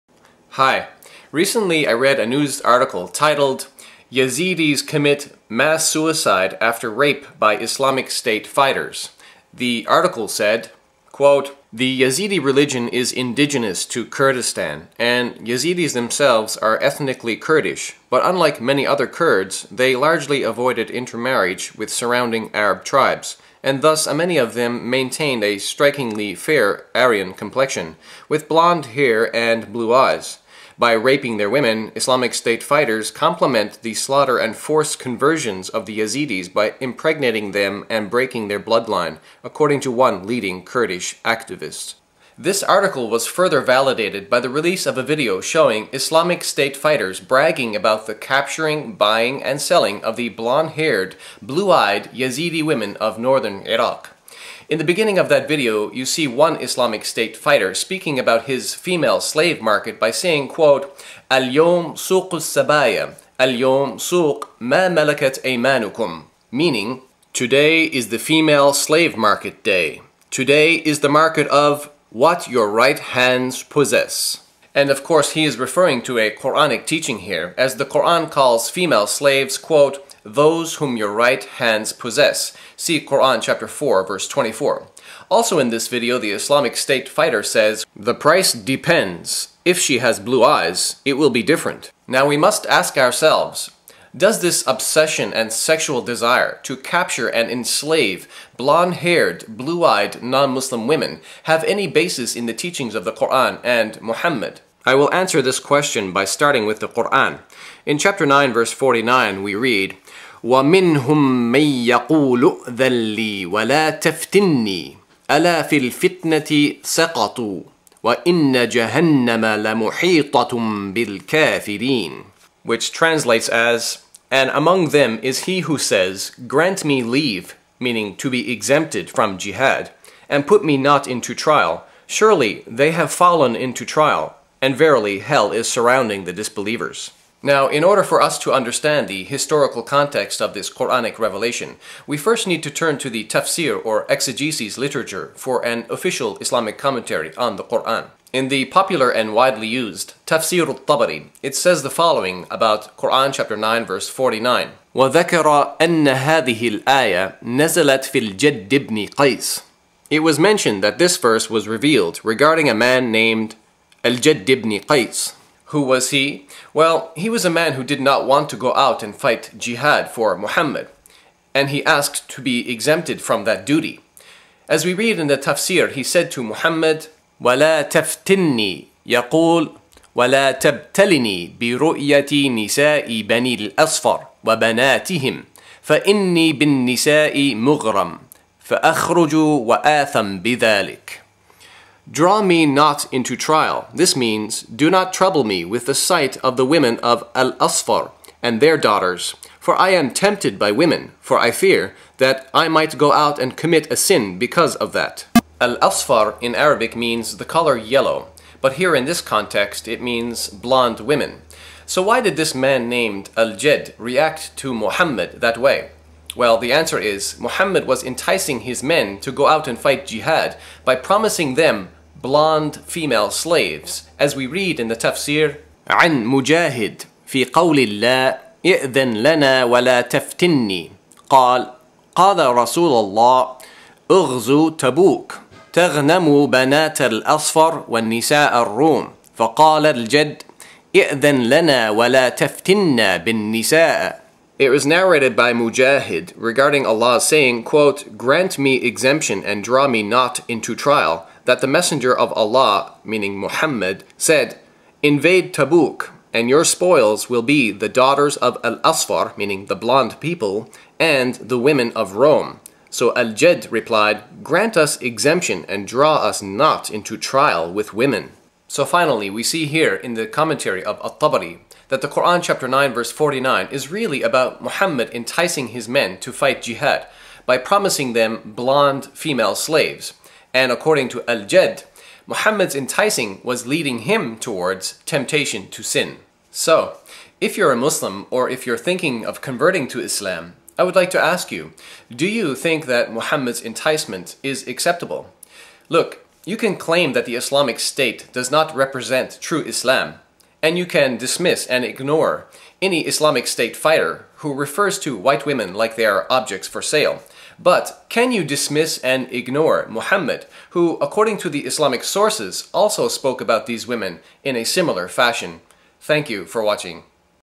Muhammad and Blonde Women
A shocking explanation of Qur’an 9:49.

HERE IS THE VIDEO: "ISIS Impregnate Yazidi girls to destroy their blonde-haired, blue-eyed bloodline"